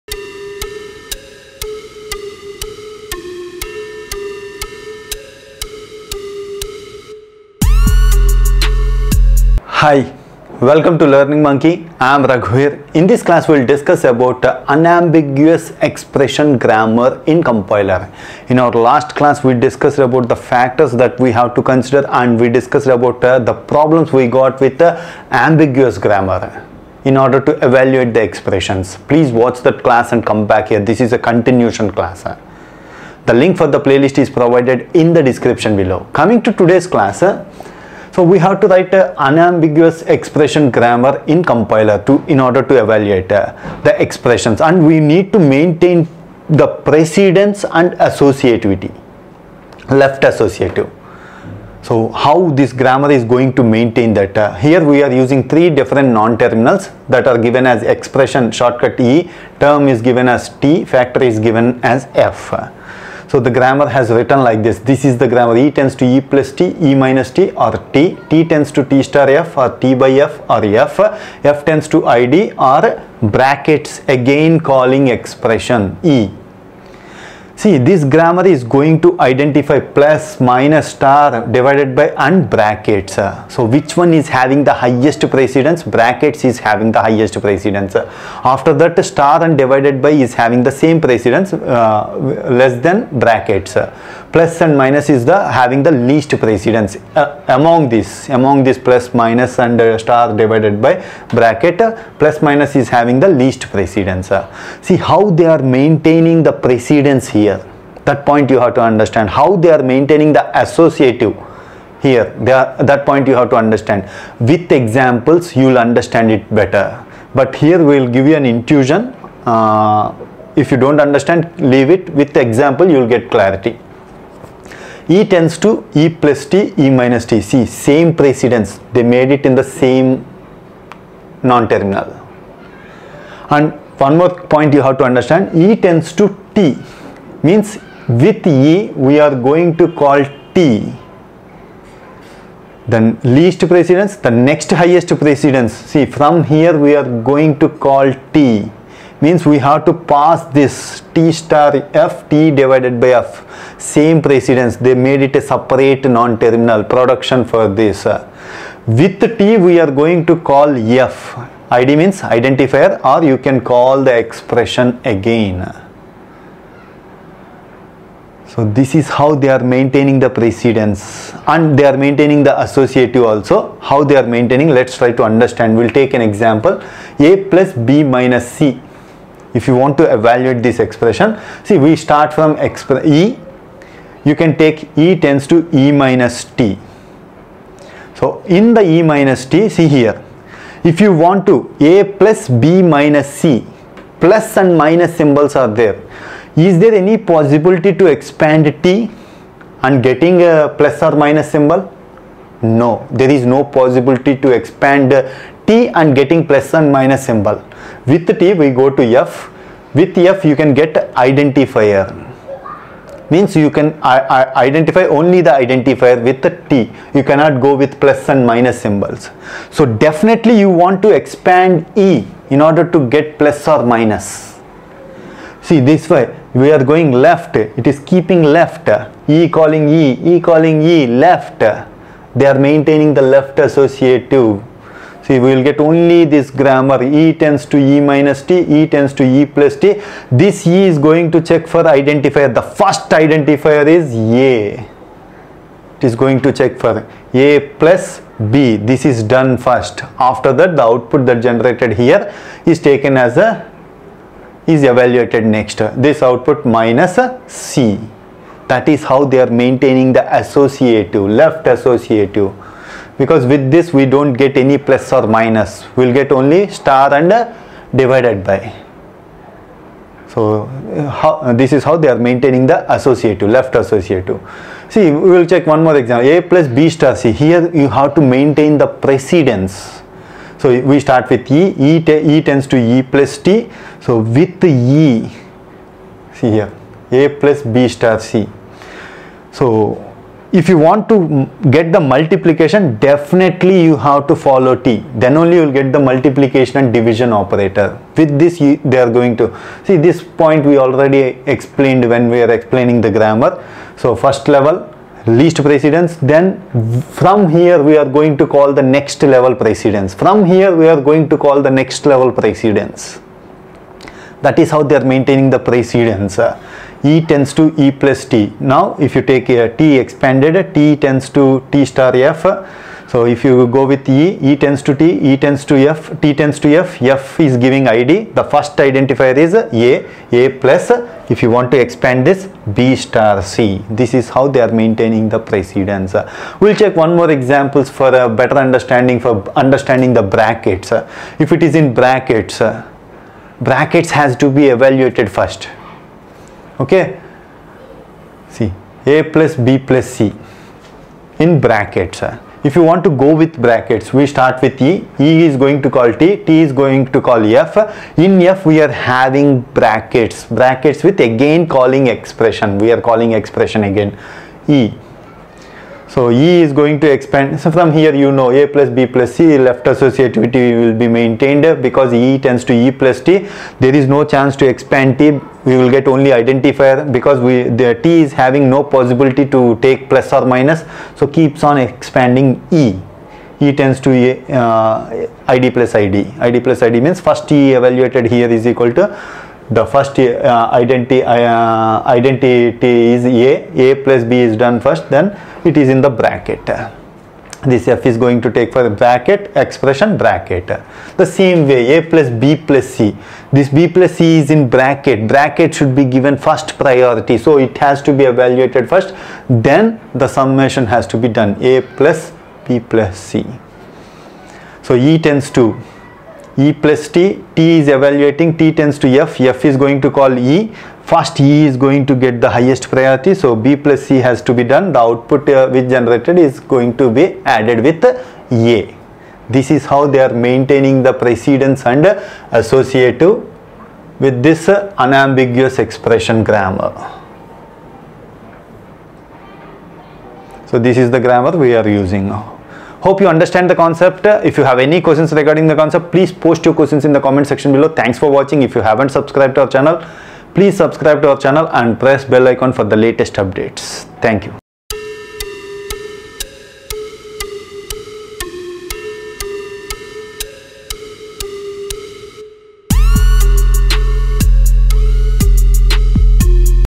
Unambiguous Expression Grammar in Compiler || Lesson 11 || Compiler Design || Learning Monkey
Unambiguous Expression Grammar in Compiler
In this class, We discuss Unambiguous Expression Grammar in Compiler.
The reader should have prior knowledge of ambiguous expression grammar. Click Here.
In our last class, we discussed the expression grammar should maintain precedence and associative conditions.
The below example grammar will maintain the precedence and associative conditions.
The three non-terminals used are Expression E, Term T, and Factor F.
The grammar starting symbol is E.
In the expression grammar, we use ‘+,’ ‘-, ‘‘*,’ ‘/,’ and ‘(. ‘
The brackets have the highest precedence.
The second highest precedence is for * and /.
The least precedence is for + and -.
How are we maintaining the precedence condition?
The + and – have the same precedence, giving the same non-terminal E.
Similarly, * and / given to the same non-terminal T.
The least precedence + and – used in non-terminal E and E calling T.
From the non-terminal E, we are moving to Non-terminal T.
Similarly, the non-terminal T is called F.
The last production F
We need to pass E to go for T and use * and / symbol. With this, we are maintaining precedence.
The below example helps the reader to understand the precedence condition better.
Example: a + b * c
The first production is E – E + T.
To take b * c, we should expand T. Without T; We can not get the symbol * and /.
We are maintaining precedence by giving step-wise in grammar.
The below example shows the associative condition.
First, we expand E – E – T.
The non-terminal T will check the id.
To take +, we should expand E., i.e., we should expand the left side non-terminal.
So we are maintaining left-associative with the expansion of left non-terminal for the same precedence.
The below examples show the precedence for brackets.
Example: (a+b+c) and a+(b+c)
The below example shows the derivation tree for (a+b+c) and a+(b+c)